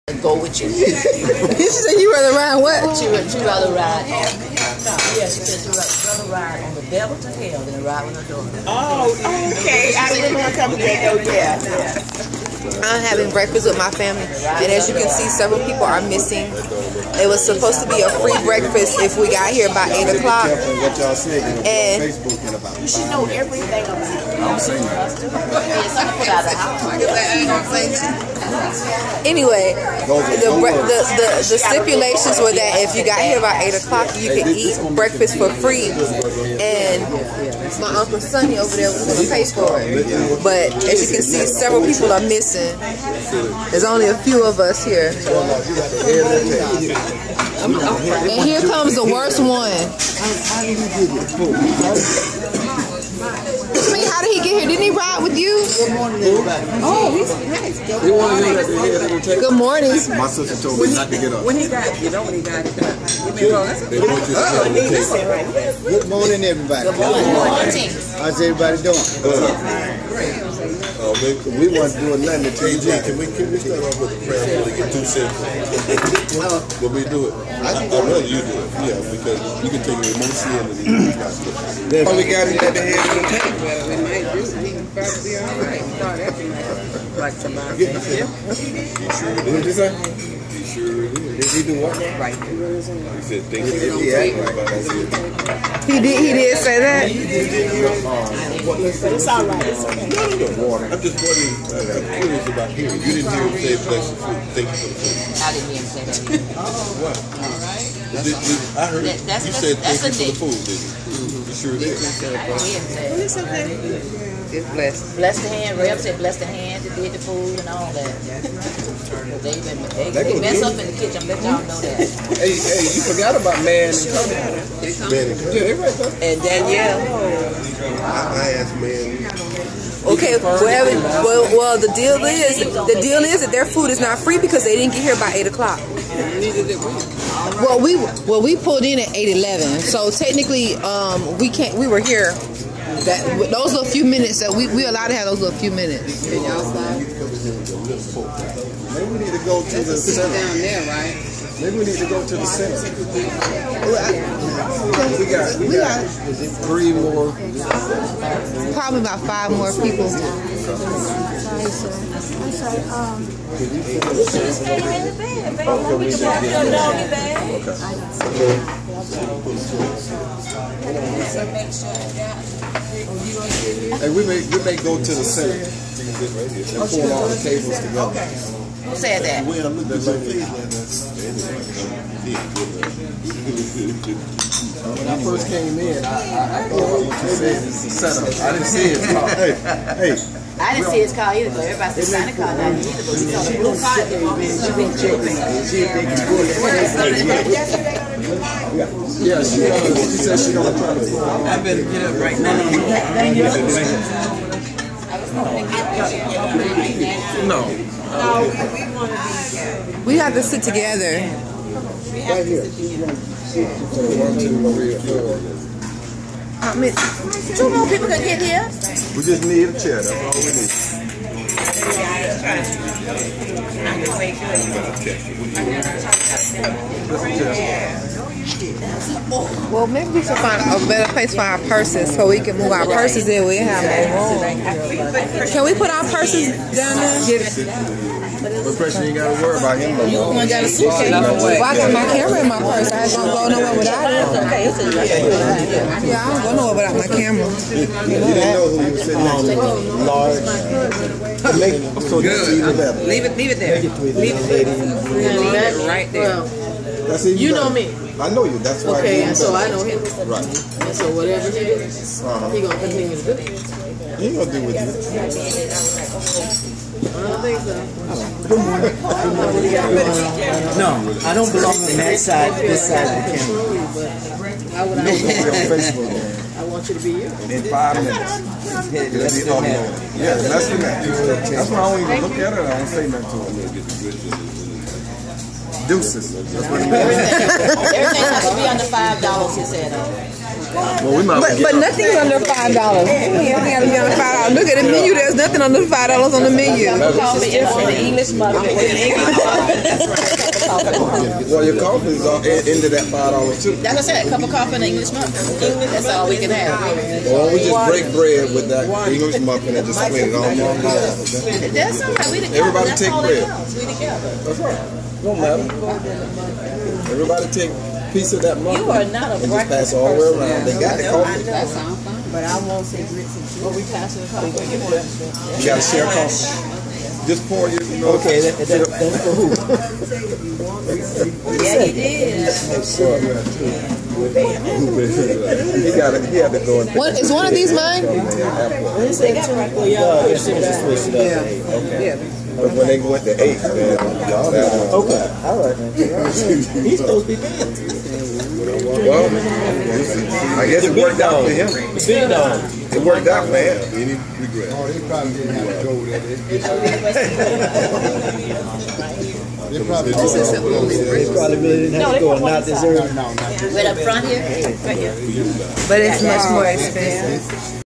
Holloway Family breakfast 2013 - Hattie Mae Branch Holloway service wknd
a few of us having breakfast early Friday morning bc uncle Sonny said he was paying for it.